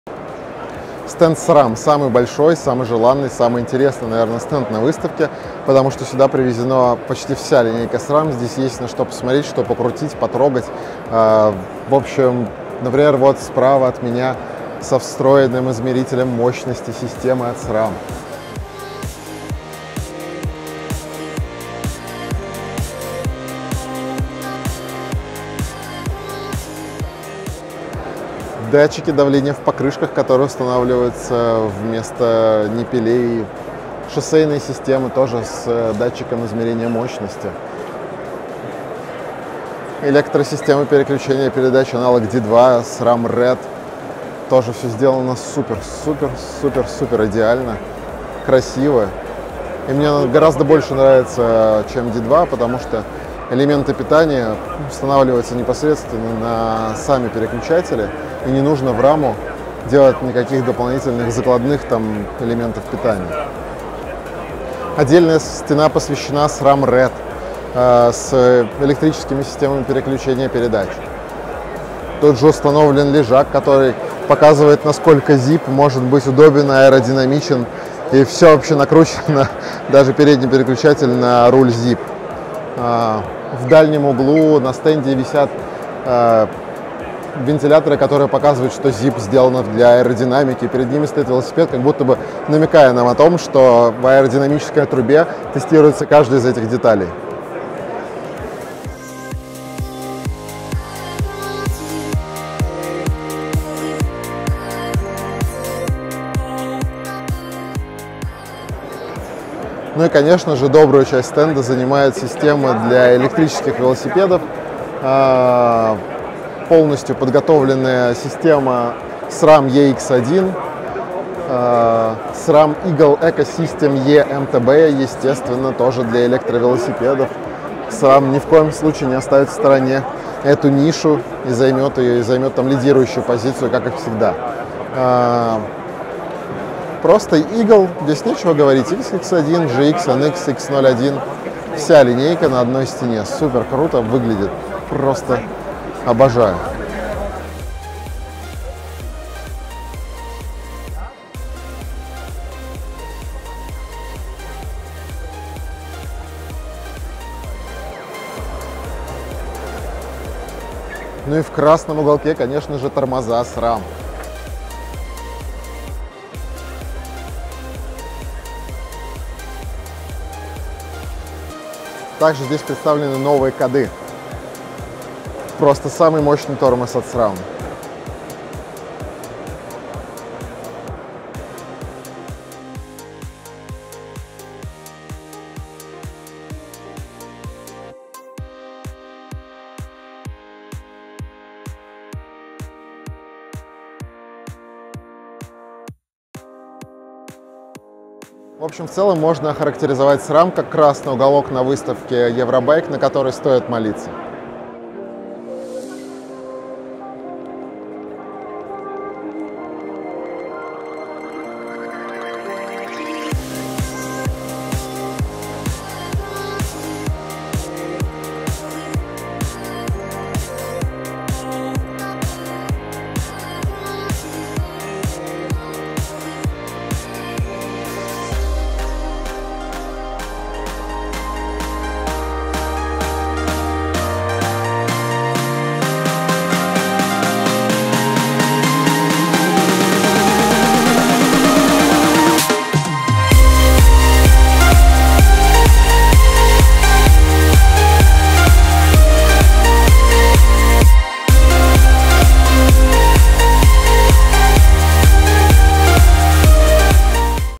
EuroBike 2018 | Sram
SRAM Corporation - это американская компания, которая является одним из крупнейших в мире производителей оборудования для велосипедов.

Компания выпускает полный спектр оборудования для горных и шоссейных велосипедов: вилки и амортизаторы, оборудование переключения передач, тормозные системы и др. Продукция поставляется под торговыми марками SRAM, RockShox, Truvativ, Avid, Zipp и др.

Оборудование SRAM и дочерних брендов активно используется профессионалами и любителями шоссейного велоспорта, в том числе командами Мирового тура.